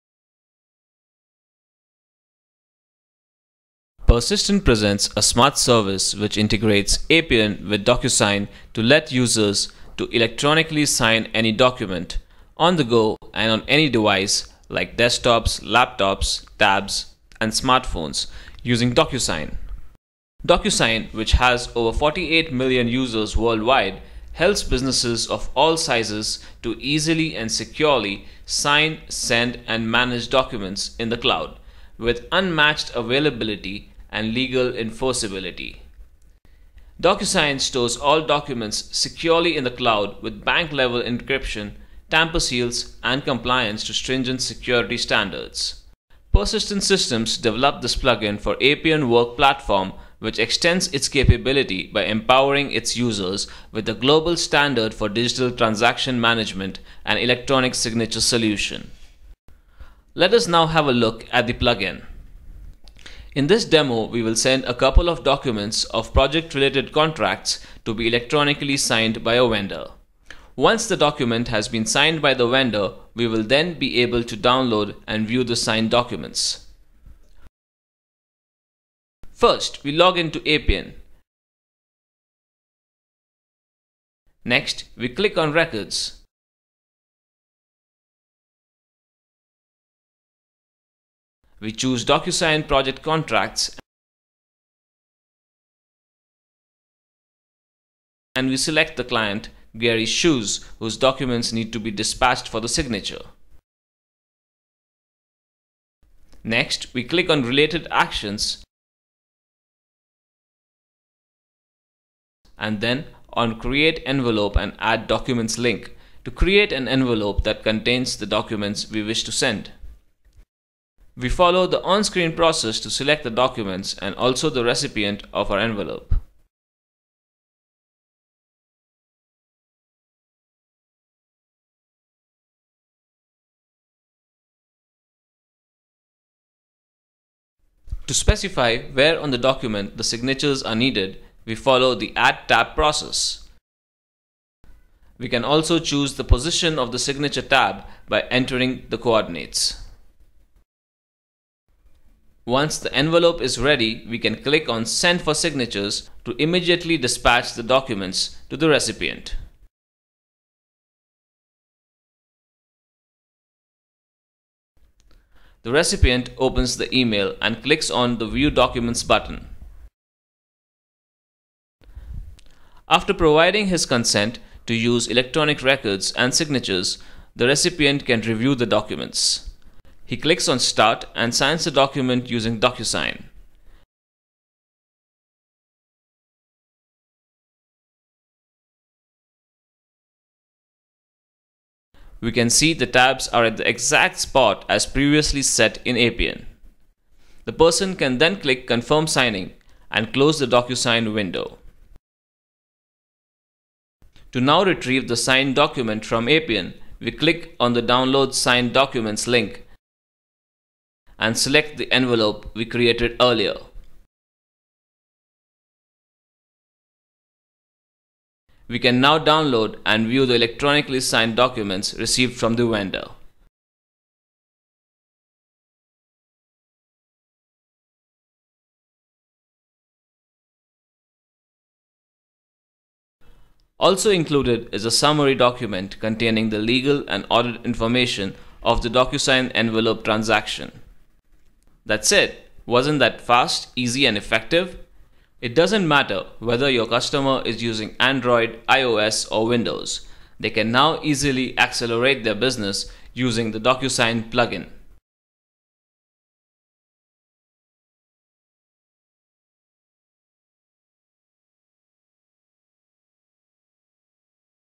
DocuSign plugin for Appian
A smart service which integrates Appian with DocuSign to let users to electronically sign any document on the go.